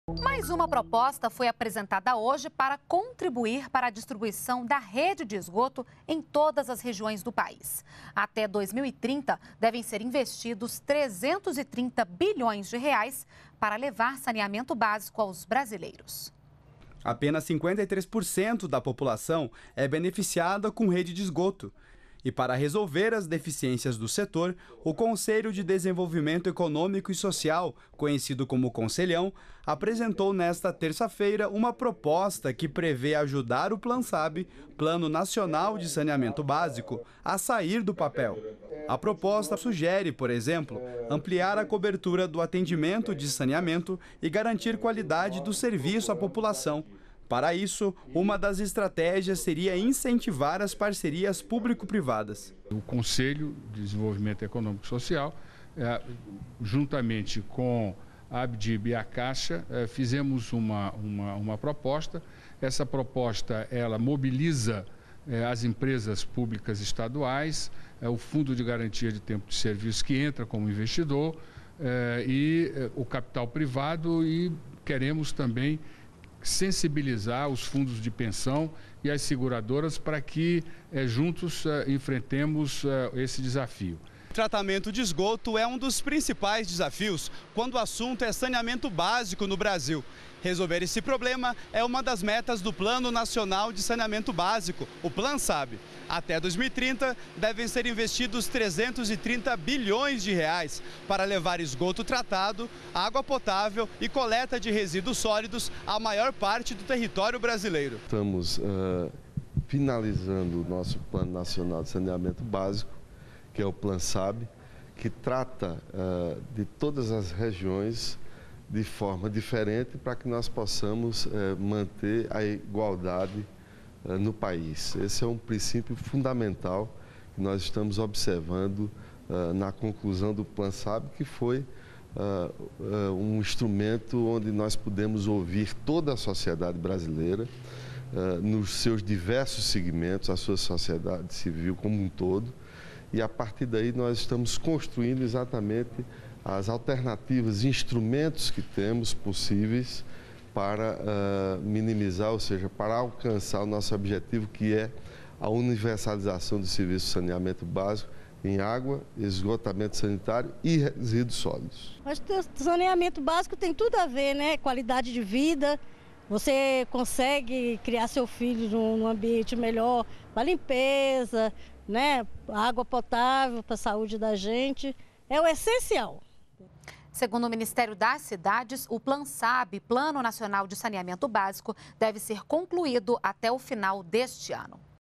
Até 2030, Plansab deve investir R$ 330 bilhões em saneamento básico no país
NBR NOTÍCIAS - 12.03.13: O Conselho de Desenvolvimento Econômico e Social (CDES) apresentou, nesta terça-feira (12), uma proposta que ajude a colocar em prática o Plano Nacional de Saneamento Básico (Plansab). Entre as ações está contribuir para a distribuição de rede de esgoto em todas as regiões do país. Até 2030, devem ser investidos R$ 330 bilhões para levar saneamento básico aos brasileiros. Segundo o Ministério das Cidades, o Plansab deve ser concluído até o final deste ano.